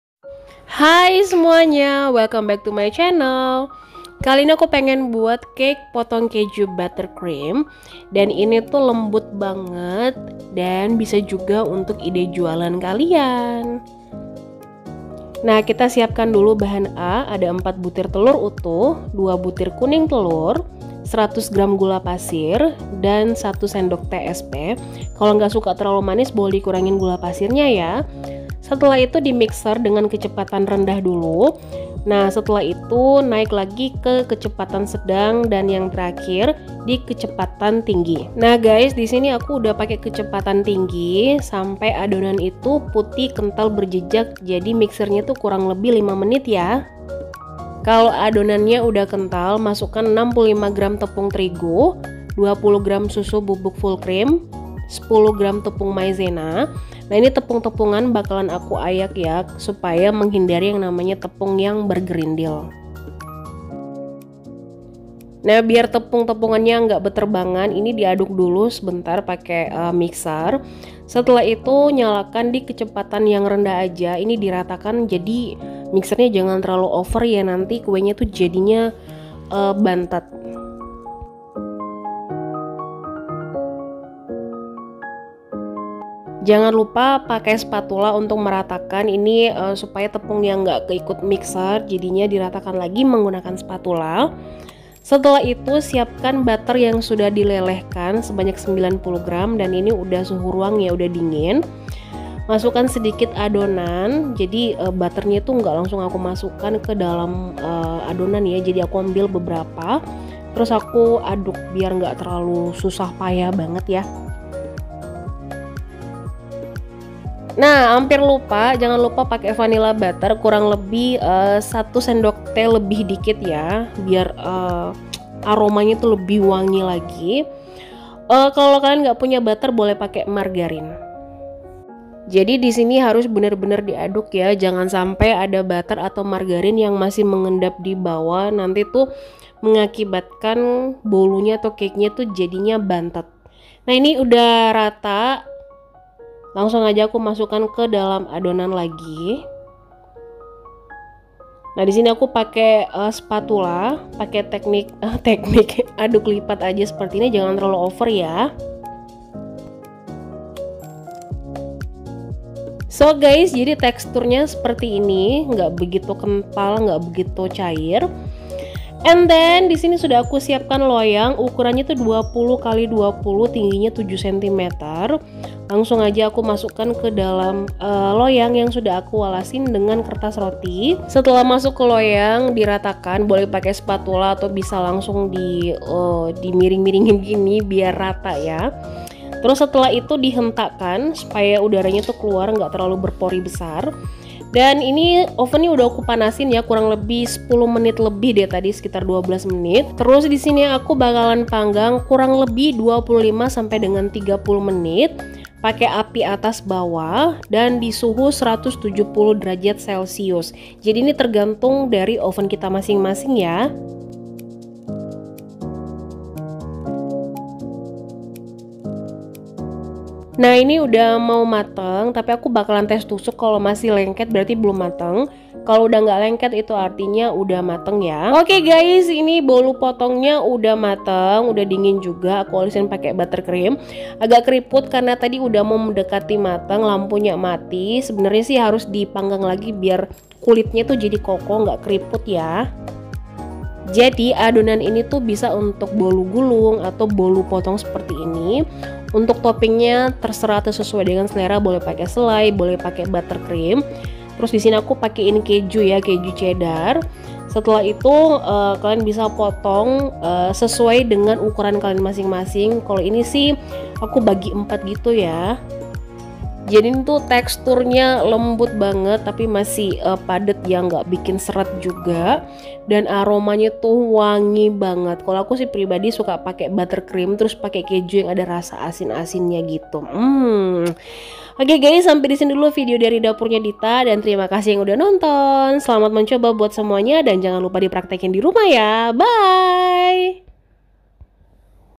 BOLU POTONG KEJU | SUDAHLAH, INI ENAK POKOKNYA !!!
Bahan A :
4btr telur utuh
2btr kuning telur
100gr gula pasir
1sdt SP

Bahan B :
65gr tepung terigu
20gr susu bubuk
10gr tepung maizena

Bahan C :
90gr Butter/margarin
1sdt esens vanila